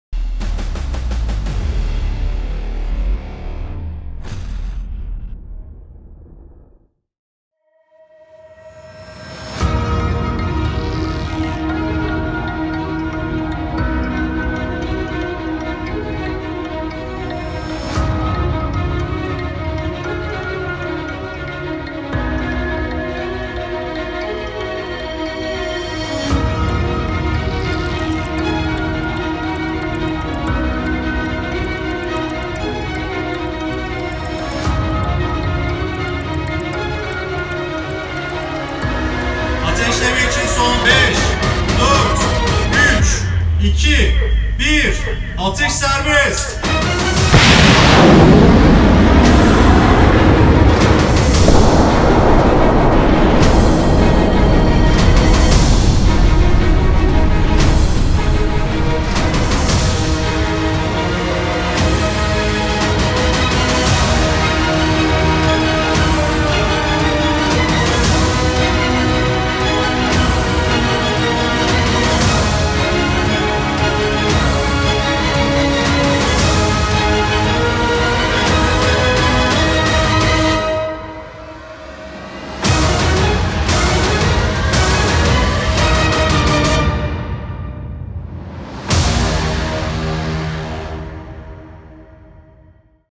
ROKETSAN | ATMACA Ataletsel Navigasyon Test Atışı [29.09.2020]
SSB Başkanı Sn. Prof. Dr. İsmail DEMİR; Ülkemizin ilk deniz füzesi Atmaca’nın en zor testlerinden birini geride bıraktık.
Atmaca, GPS’den bağımsız, kendi iç ataletsel navigasyon ünitesiyle atıldı ve başarılı oldu. Son olarak harp başlığıyla yapacağımız teste hazırlanıyoruz, yakında seri üretim başlıyor.